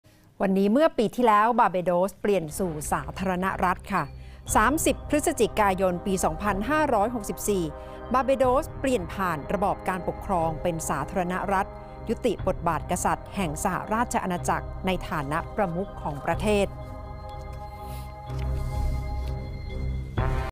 วันนี้เมื่อปีที่แล้ว "บาร์เบโดส" เปลี่ยนสู่สาธารณรัฐ
30  พฤศจิกายน 2564  บาร์เบโดส เปลี่ยนผ่านระบอบการปกครองเป็นสาธารณรัฐ ยุติบทบาทกษัตริย์แห่งสหราชอาณาจักร ในฐานะประมุขของประเทศ